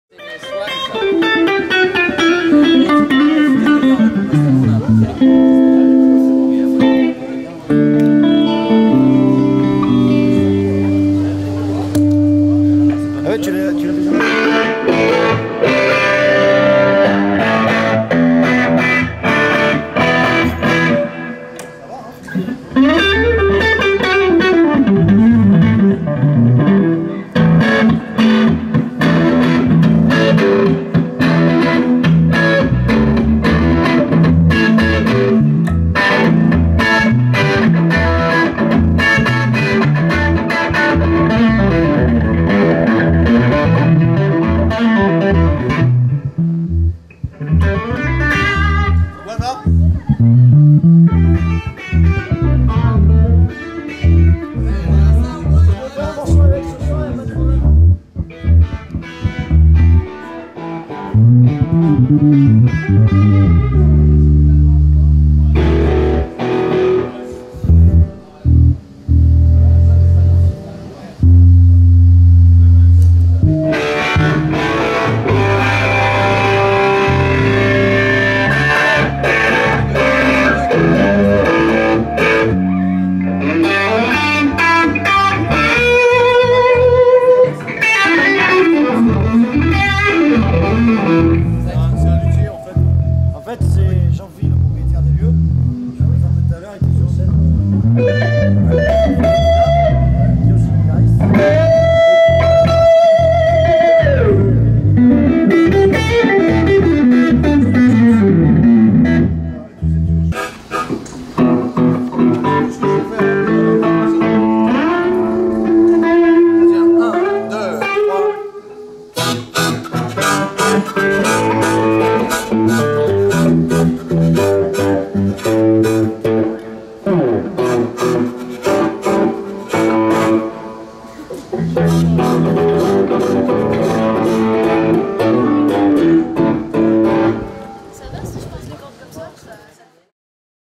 Les guitares à corps métallique Jean-Phillipe Fally
Les guitares à corps métallique Jean-Phillipe Fally dans les mains de Philippe Hervouët, Philippe Gonnand et Rachelle Plas.

Filmé pendant le festival Rhinoférock 2014, à Pernes les Fontaines le 26 juillet 2014.